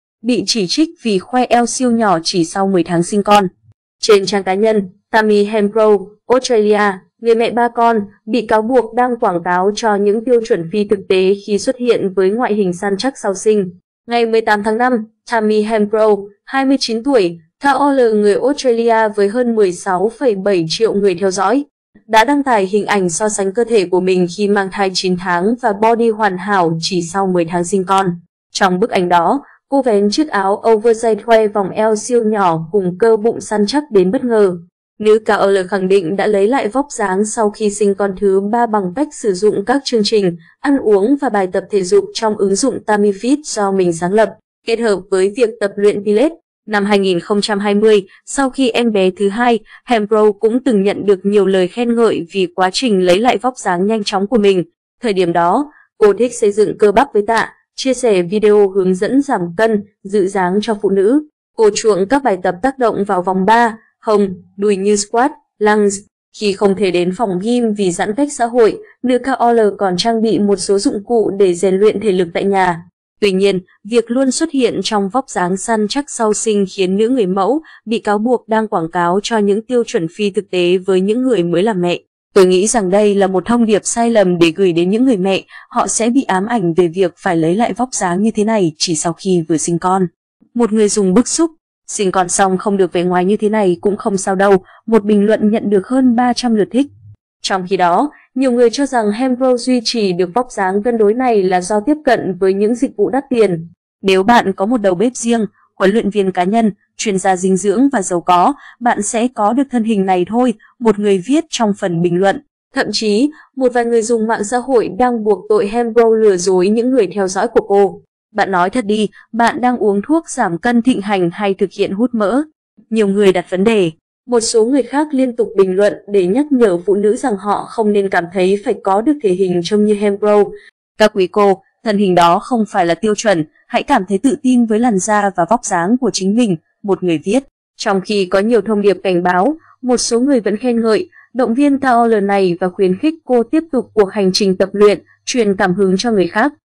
Bị chỉ trích vì khoe eo siêu nhỏ chỉ sau 10 tháng sinh con
Trên trang cá nhân, Tammy Hembrow (Australia), người mẹ 3 con, bị cáo buộc đang quảng cáo cho những tiêu chuẩn phi thực tế khi xuất hiện với ngoại hình săn chắc sau sinh."Tôi nghĩ rằng đây là một thông điệp sai lầm để gửi đến những người mẹ, họ sẽ bị ám ảnh về việc phải lấy lại vóc dáng như thế này chỉ sau khi vừa sinh con", một người dùng bức xúc.
"Sinh con xong không được vẻ ngoài như thế này cũng không sao đâu", một bình luận nhận được hơn 300 lượt thích.
"Nếu bạn có một đầu bếp riêng, huấn luyện viên cá nhân, chuyên gia dinh dưỡng và giàu có, bạn sẽ có được thân hình này thôi", một người viết trong phần bình luận.
Thậm chí, một vài người dùng mạng xã hội đang buộc tội Hembrow lừa dối những người theo dõi của cô.
Tuy nhiên, việc luôn xuất hiện trong vóc dáng săn chắc sau sinh khiến nữ người mẫu bị cáo buộc đang quảng cáo cho những tiêu chuẩn phi thực tế với những người mới làm mẹ.
Một số người khác liên tục bình luận để nhắc nhở phụ nữ rằng họ không nên cảm thấy phải có được thể hình trông như Hembrow.
Trong khi đó, nhiều người cho rằng Hembrow duy trì được vóc dáng cân đối này là do tiếp cận với những dịch vụ đắt tiền.
 Bức ảnh gây tranh cãi mà Tammy Hembrow đăng tải trên mạng xã hội. Ảnh: Tammy Hembrow.
Trong khi có nhiều thông điệp cảnh báo, một số người vẫn khen ngợi, động viên KOL này và khuyến khích cô tiếp tục cuộc hành trình tập luyện, truyền cảm hứng cho người khác.
Ngày 18/5, Tammy Hembrow, 29 tuổi, KOL người Australia với hơn 16,7 triệu người theo dõi, đã đăng tải hình ảnh so sánh cơ thể của mình khi mang thai 9 tháng và body hoàn hảo chỉ sau 10 tháng sinh con.
Trong bức ảnh đó, cô vén chiếc áo oversized khoe vòng eo siêu nhỏ cùng cơ bụng săn chắc đến bất ngờ. Nữ KOL khẳng định đã lấy lại vóc dáng sau khi sinh con thứ ba bằng cách sử dụng các chương trình ăn uống và bài tập thể dục trong ứng dụng TammyFit do mình sáng lập, kết hợp với việc tập luyện pilates.
"Bạn nói thật đi, bạn đang uống thuốc giảm cân thịnh hành hay thực hiện hút mỡ?", nhiều người đặt vấn đề.
"Các quý cô, thân hình đó không phải là tiêu chuẩn, hãy cảm thấy tự tin với làn da và vóc dáng của chính mình”, một người viết.
Năm 2020, sau khi em bé thứ hai, Hembrow cũng từng nhận được nhiều lời khen ngợi vì quá trình lấy lại vóc dáng nhanh chóng của mình.
Thời điểm đó, cô thích xây dựng cơ bắp với tạ, chia sẻ video hướng dẫn giảm cân, giữ dáng cho phụ nữ. Cô chuộng các bài tập tác động vào vòng 3, hông, đùi như squats, lunges. Khi không thể đến phòng gym vì giãn cách xã hội, nữ KOL còn trang bị một số dụng cụ để rèn luyện thể lực tại nhà.